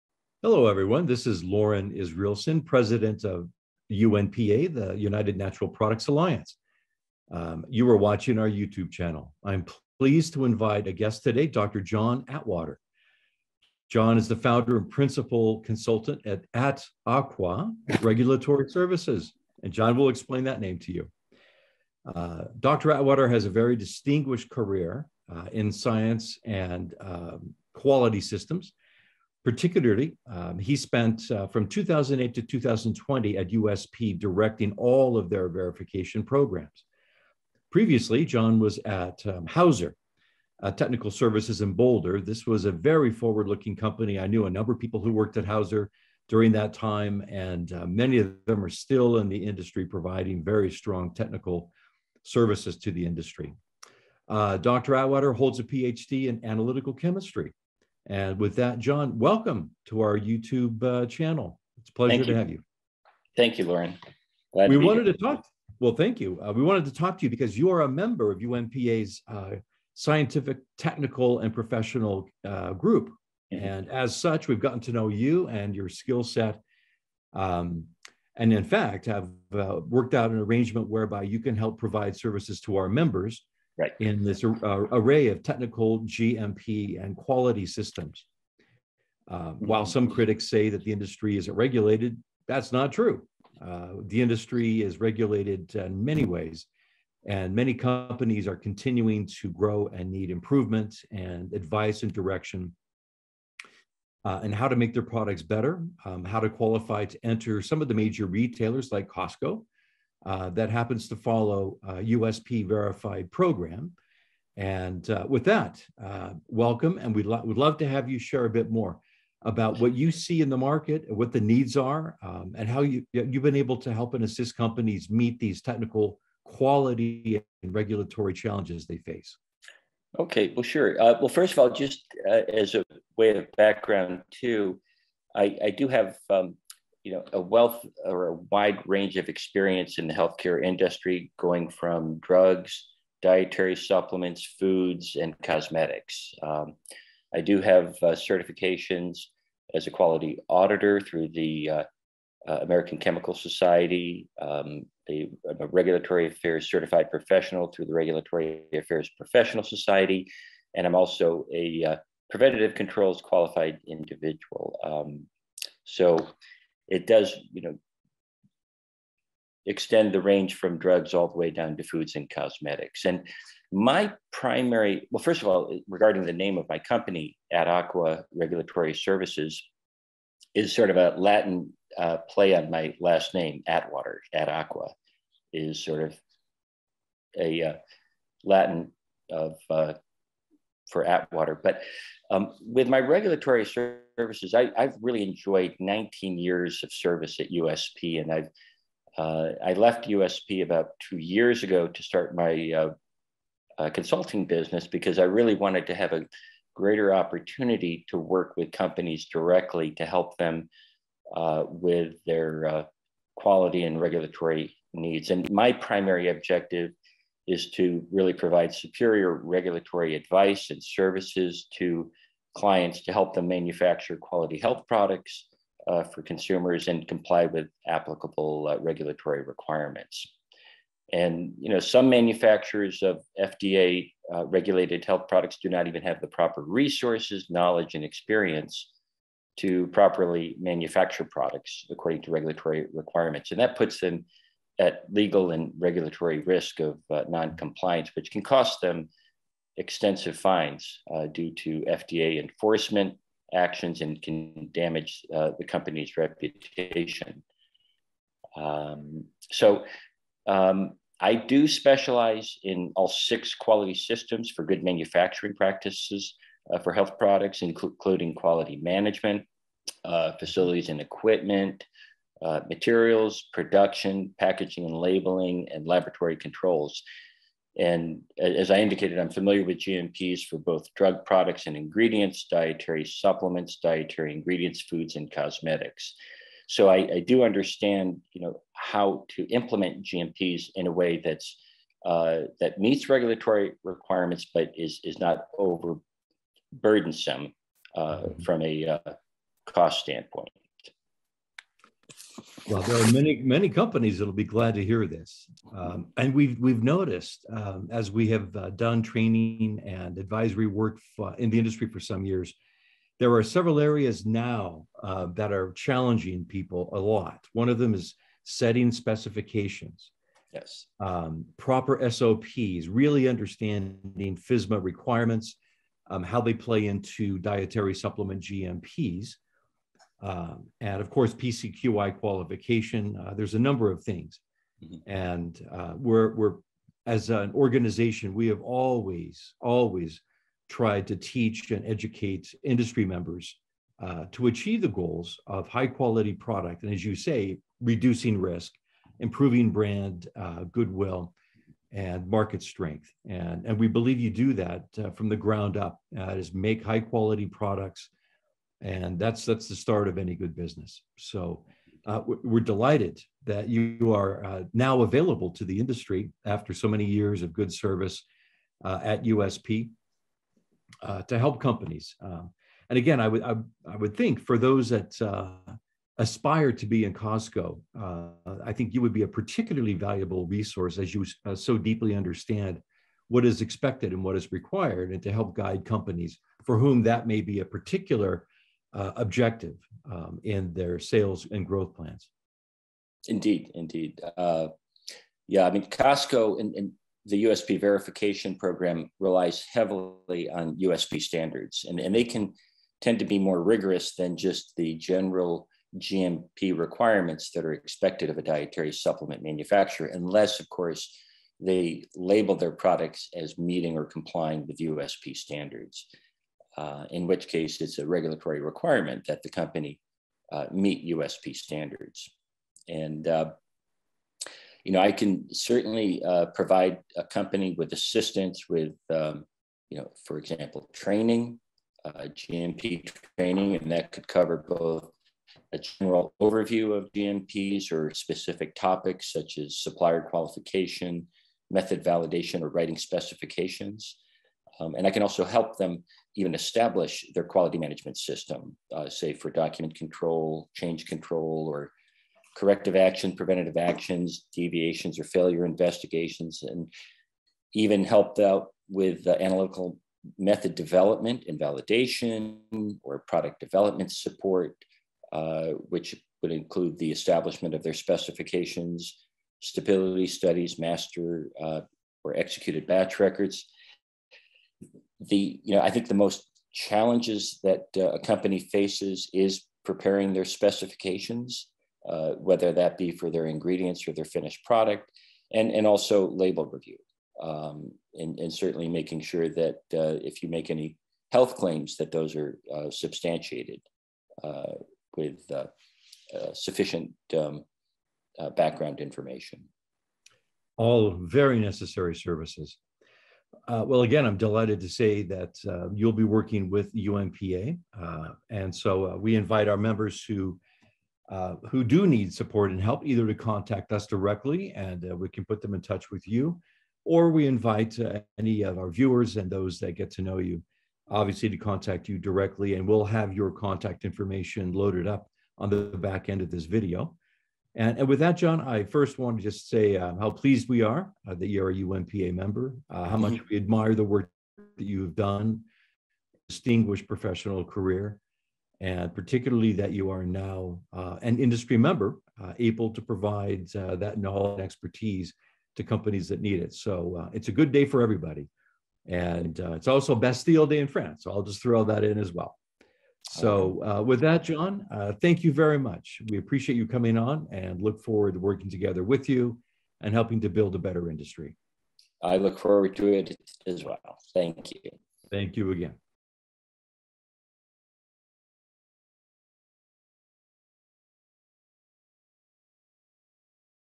UNPA Interview | John Atwater - Regulatory Consultant - Valuable Resource for UNPA Members
Loren introduces John Atwater as our primary regulatory consultant, a valuable resource for all UNPA Members.

John B. Atwater
Principal Consultant
AtAqua Regulatory Services